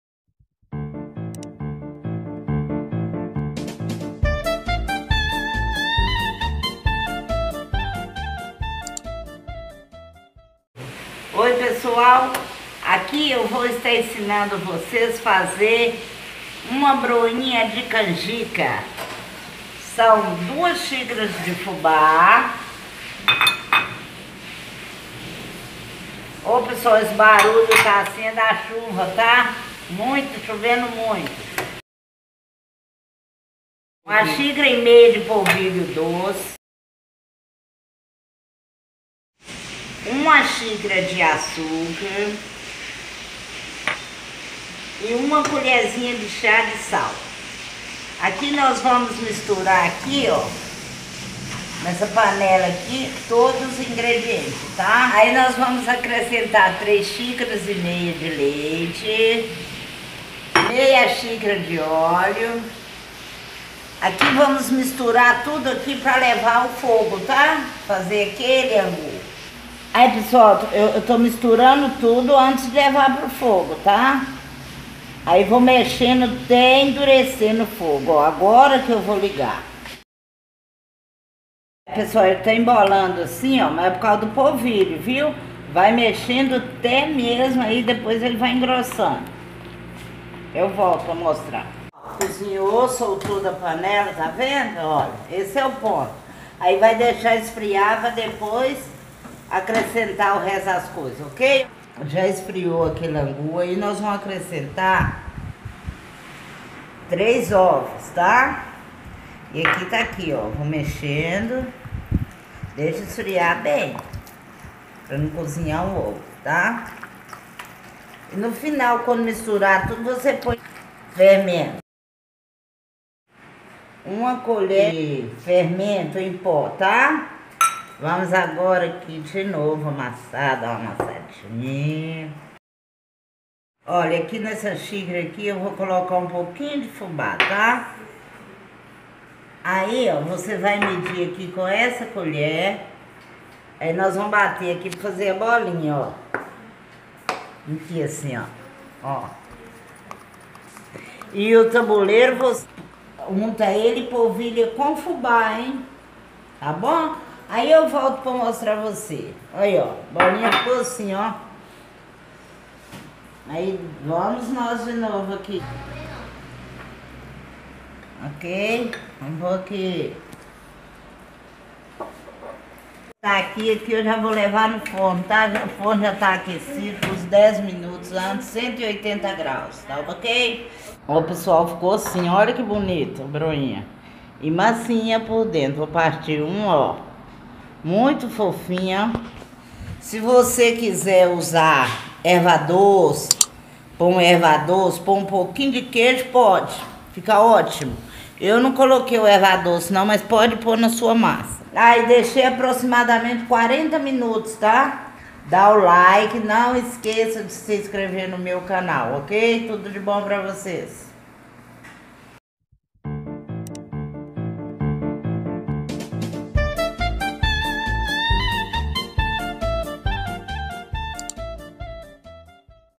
Broinha de Polvilho Doce com Fubá - Delícias da Dora
Hoje faço Broinha de Polvilho Doce com Fubá. Fica uma delícia para o lanche.

Acompanhem o modo de preparo no vídeo.

Ingredientes:

2 Xic de Fubá
1Xic e 1/2 de Polvilho Doce
1 Xic de açúcar
1 clh de chá de Sal
3 Xic e 1/2 de Leite
1/2 Xic de Óleo
3 Ovos
1clh de sopa de Fermento em pó

🍚Medida da Xícara é de 200ml
🍚Dica: Se quiser pode acrescentar Erva Doce ou Queijo Ralado
🍚Untar o Tabuleiro com Margarina e Fubá
🍚Aqueça o forno por 10 minutos antes em 180 graus.
🍚Assar aproximadamente em 40 minutos.

🥄Compartilha